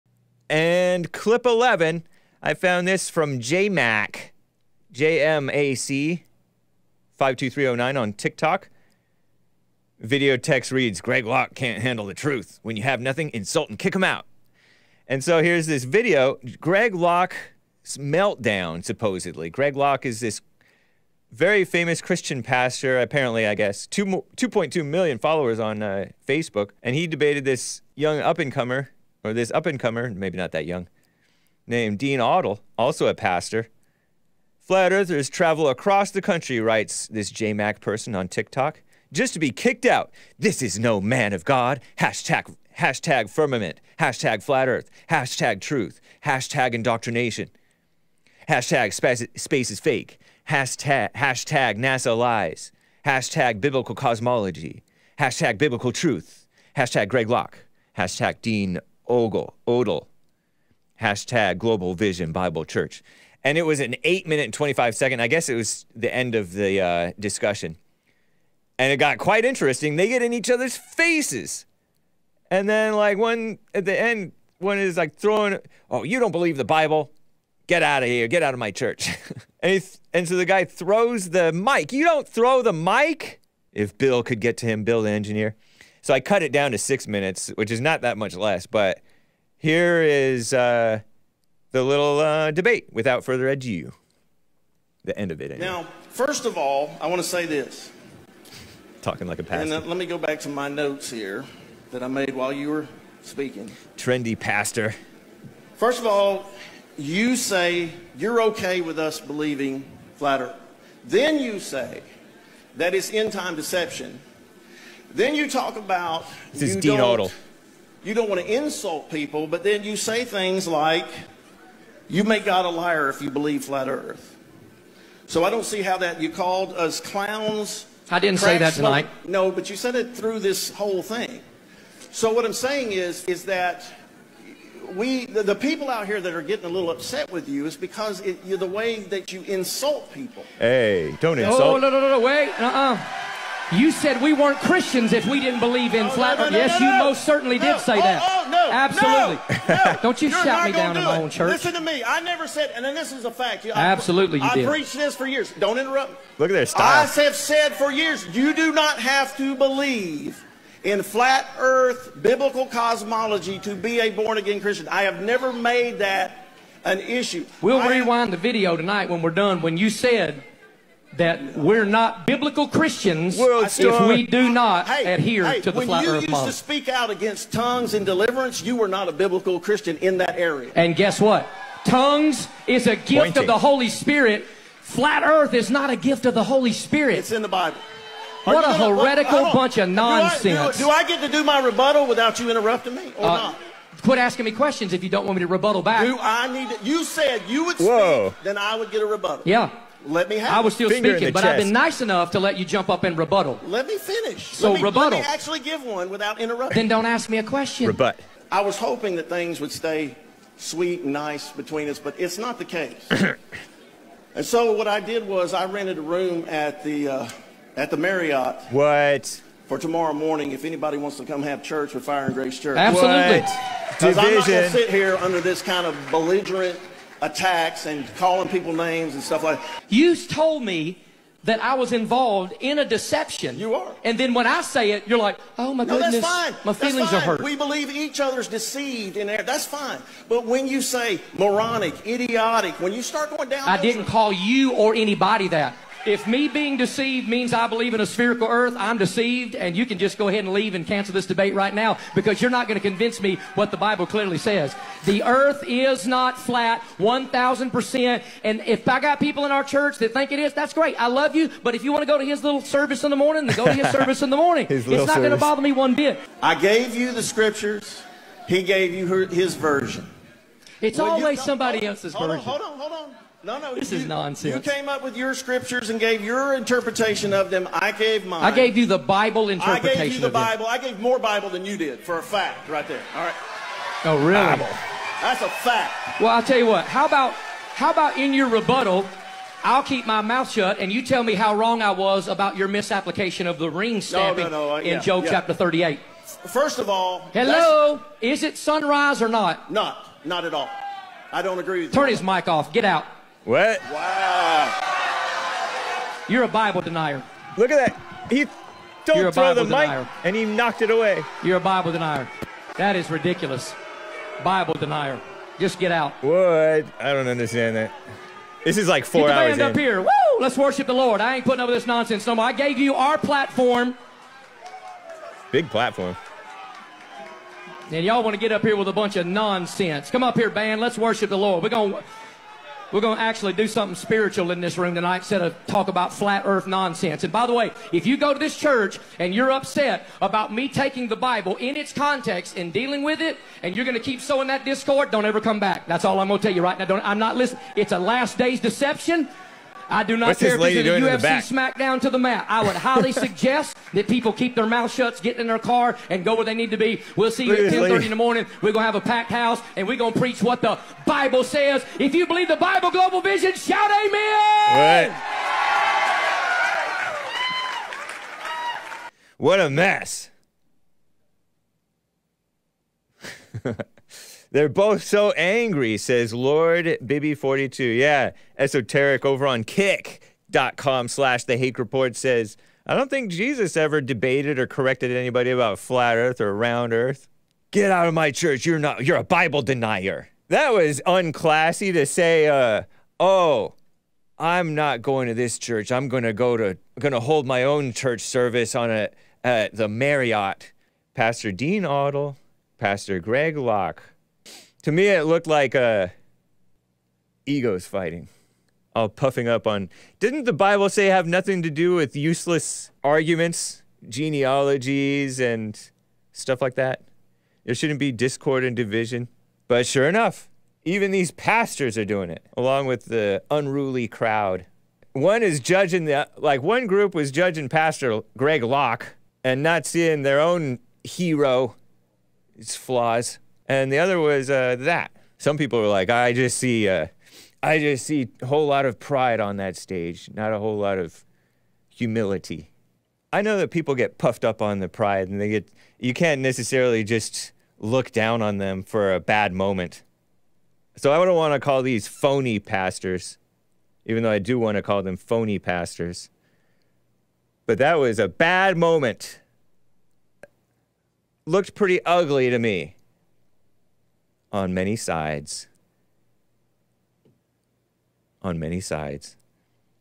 Pastors FIGHT Over Flat Earth Debate! WOW! | The Hake Report
James, host of @TheHakeReport, plays a clip and shares his commentary of two pastors (Greg Locke & Dean Odle) who previously did a live streamed debate over round & flat earth where things got heated between the two, leaving people questioning their titles as pastors.